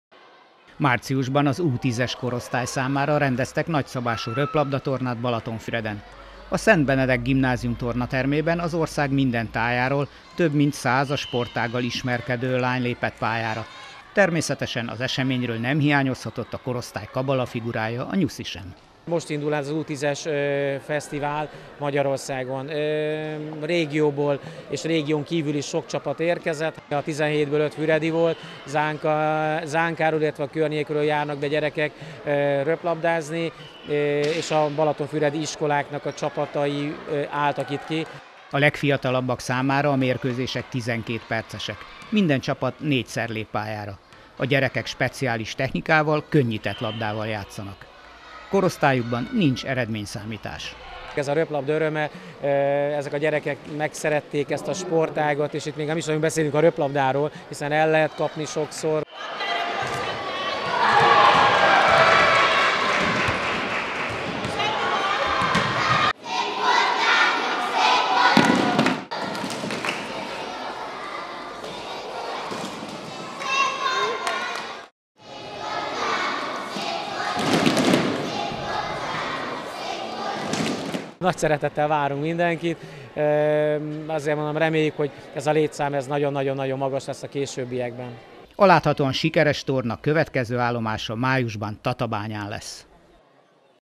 Röplabda U10-es Országos Manó Bajnokság
Három pályán tizenhét csapat részvételével rendezték meg az U10-es Országos Manó Bajnokságot Balatonfüreden. A Szent Benedek Röplabda Akadémia öt együttessel vett részt a programon. A torna lényege az önfeledt játék volt, a nap végén minden szereplő ajándékkal térhetett haza.